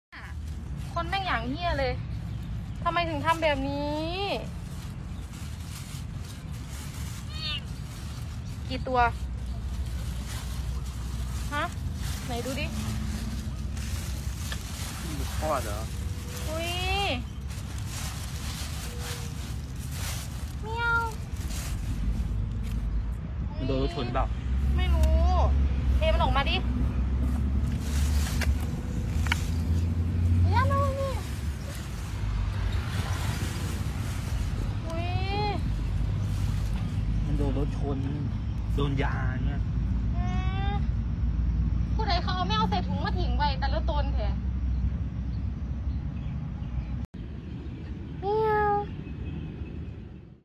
Hero passerby saves abandoned kitten from plastic bag
A hero passerby saved a kitten that was left to die after being dumped on the side of the road inside a plastic bag.

Tharinee Wisai noticed the white plastic wriggling next to a rubbish bin in Buriram province, northeast Thailand on 6 July.

Upon opening the bag, she found a wary ginger tabby kitten struggling to breathe and decided to take the feline home to live with her other pets.

“I wonder who could be so cruel to do this to a kitten. I’m relieved that we saw the cat before it was too late,” Wisai said.